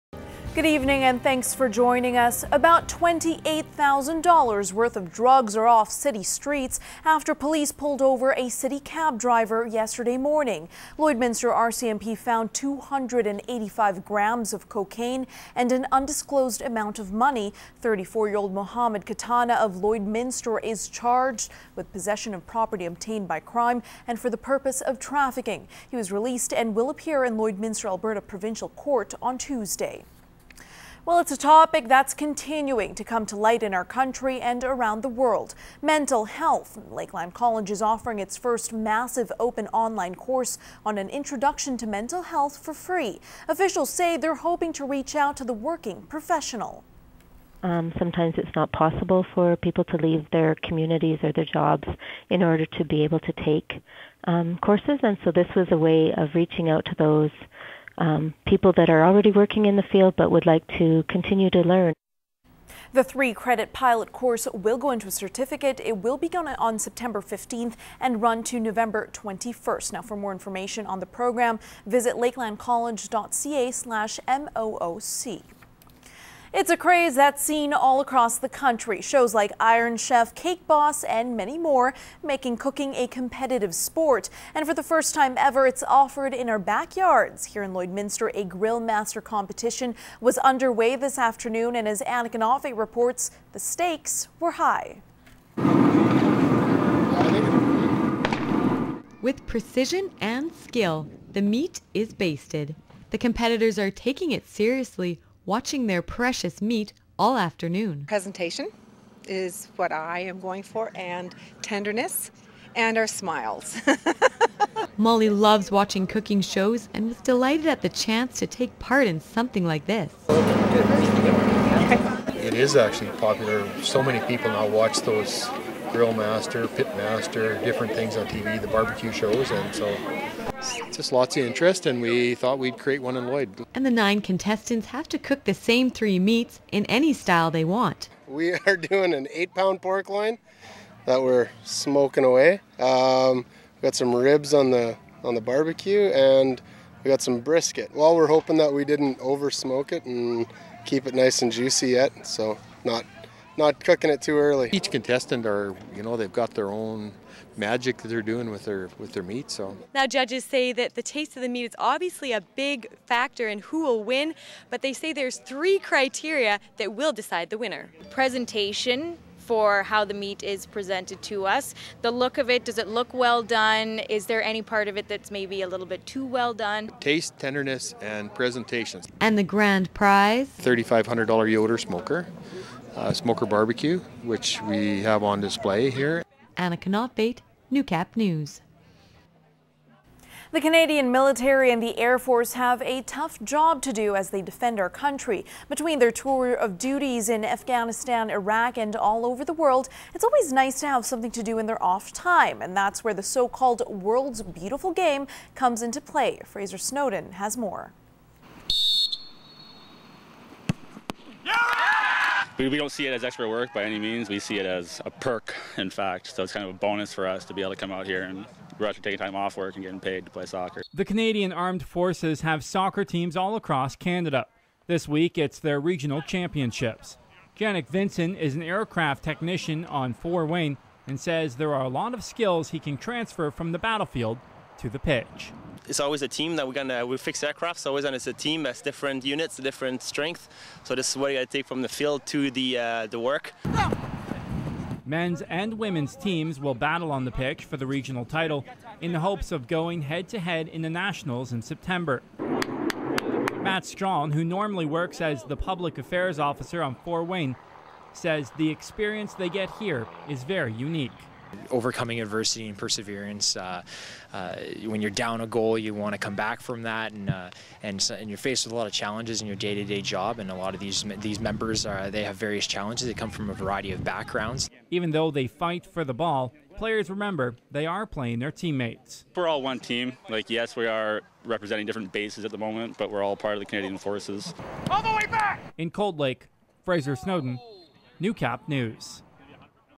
NEWCAP NEWS AUG 16, 2014
Today's news stories from Lloydminster and surrounding area including: Drug charges, Lakeland College online course, Grill Masters competition, and CFB soccer teams.